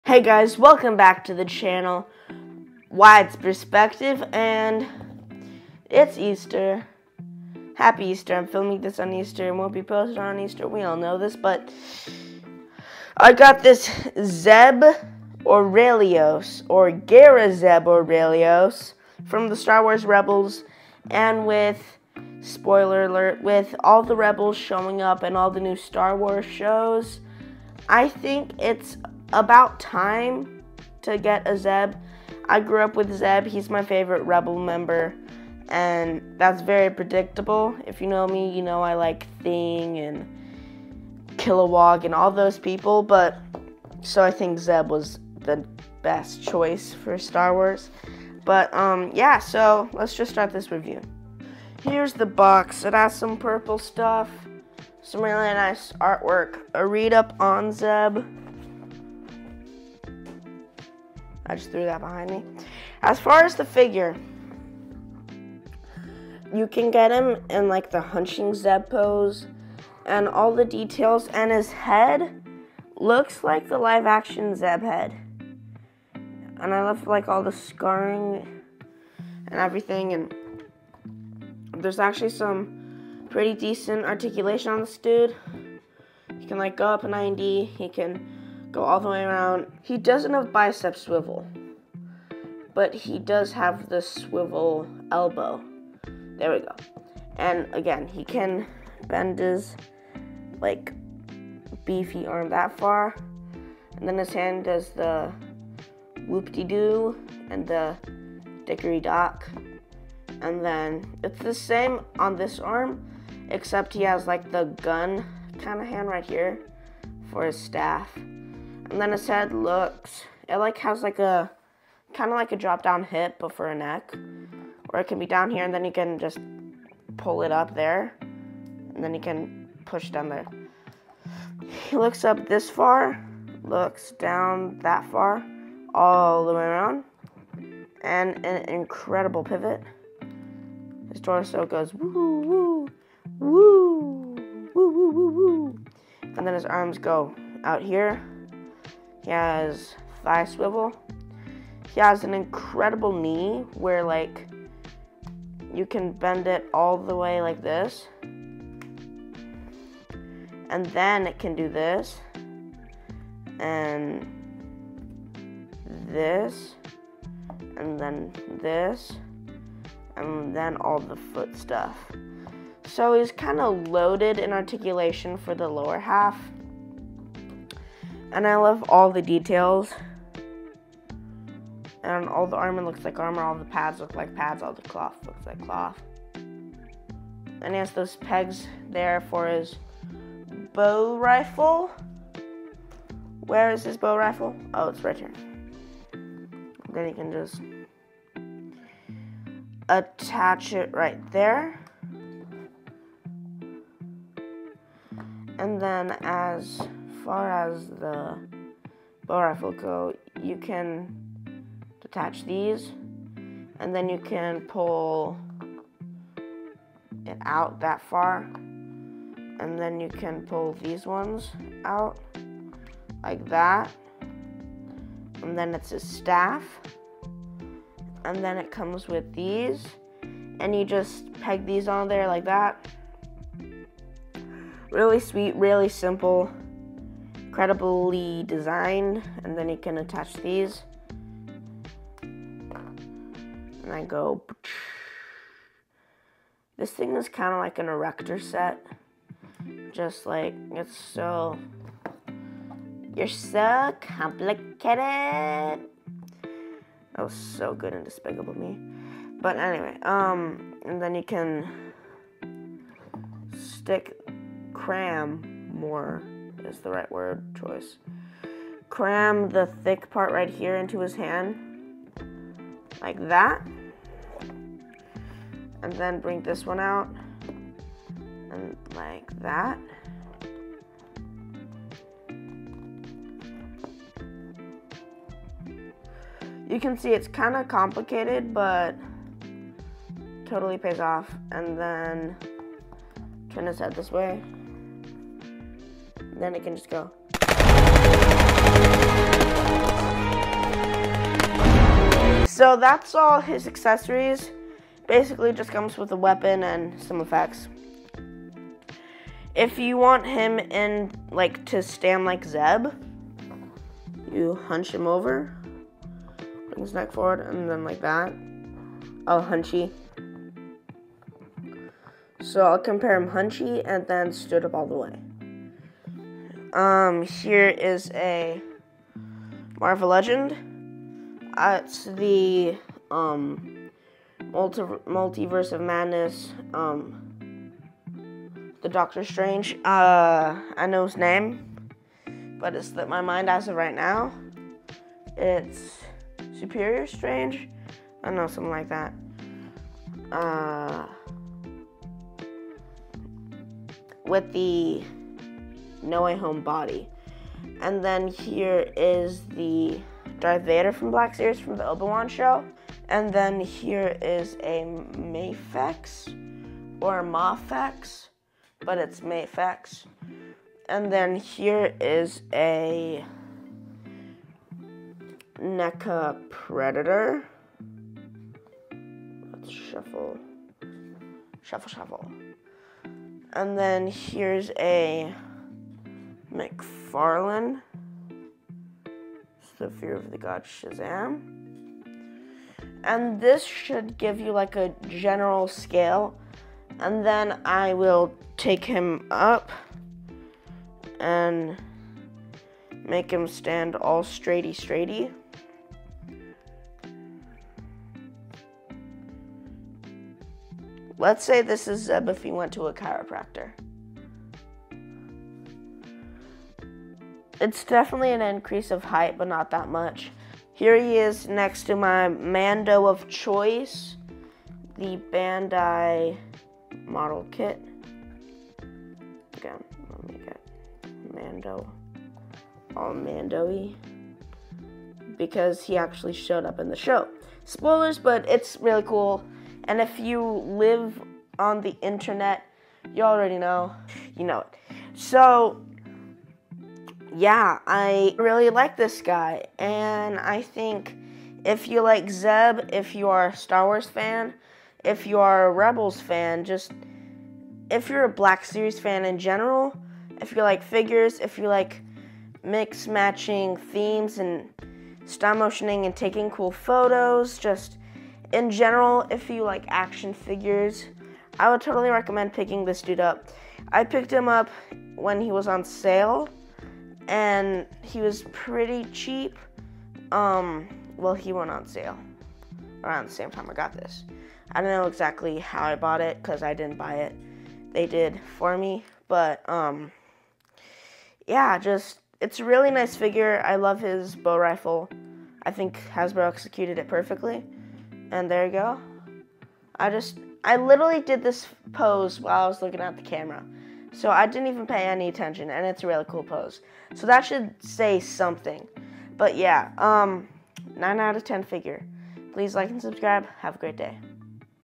Hasbro Black series Zeb figure review video
I review the Zeb figure from Hasbro see my opinion on his size accessories and details